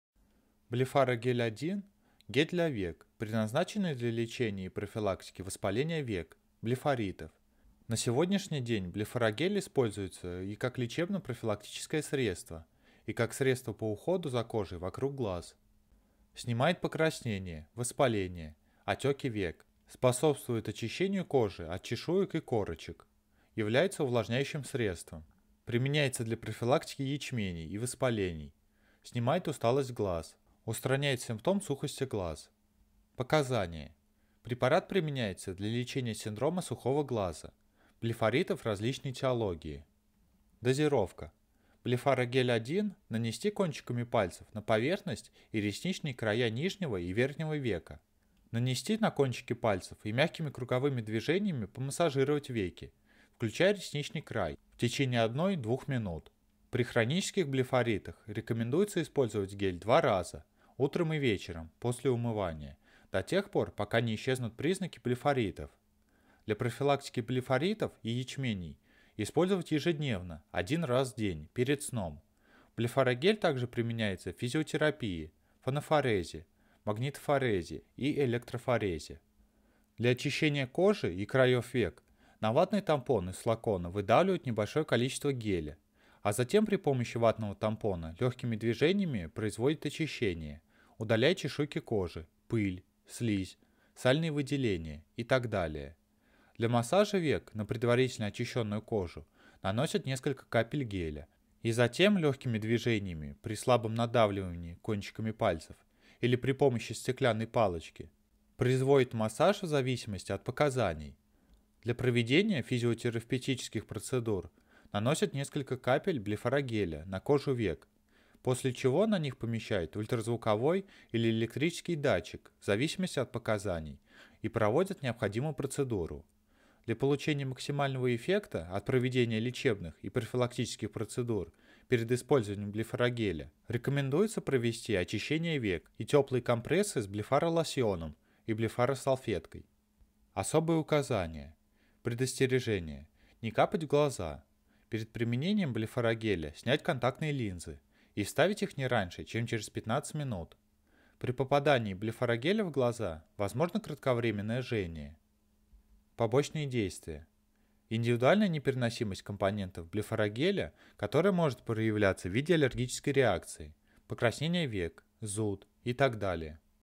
Блефарогель 1 инструкция по применению препарата. Лечение синдрома "Сухого глаза" и блефариты
Блефарогель 1 — гель для век, предназначенный для лечения и профилактики воспаления век (блефаритов). На сегодняшний день Блефарогель 1 используется и как лечебно-профилактическое средство, и как средство по уходу за кожей вокруг глаз.

-снимает покраснения, воспаления, отеки век;
-способствует очищению кожи от чешуек и корочек;
-является увлажняющим средством;
-применяется для профилактики ячменей и воспалений;
-снимает усталость с глаз, устраняет симптом сухости глаз.

О чём видео:
0:00 Описание
0:32 Показания
0:40 Дозировка
2:19 Особые условия
2:35 Побочные действия